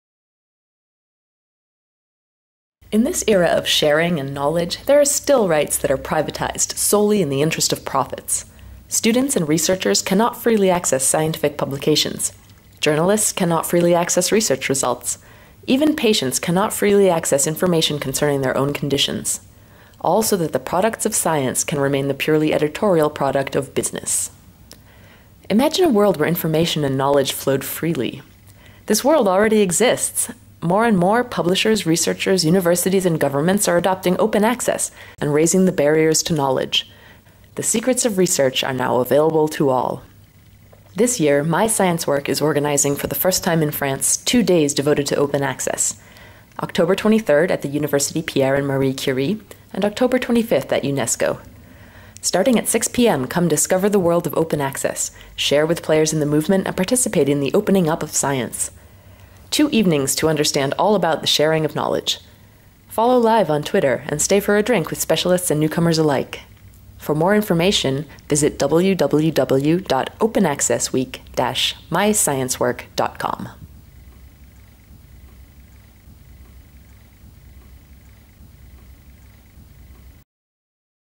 International Open Access Week 2012: Two Events in Paris!
International Open Access Week is a global event, now in its 6th year. Its goal is to increase awareness of the benefits of Open Access, and to share ideas to help Open Access become the automatic choice for scientific publication.

To learn more about why, see MyScienceWork's collection of articles discussing open access

In 2012, MyScienceWork has taken on the role of organizing, for the first time in Paris, two evenings devoted to Open Access. In partnership with UNESCO and the University Pierre and Marie Curie, MyScienceWork invites you to come learn more about this important subject, discuss with international experts, and discover ways to open up your own, or your institution's, research.

Check out the event teaser to learn more about this revolution in the system of scientific publishing and how you can get involved during Open Access Week.

If you like the video, share it, tweet it, spread the word to bring greater visibility to Open Access in France.

Save the dates:
- Tuesday, October 23, 6:00pm -- 9:00pm at University Pierre and Marie Curie (Amphitheater 25)
- Thursday, October 25, 6:00pm -- 9:00pm at UNESCO

for the full program and details.

We look forward to seeing you at International Open Access Week!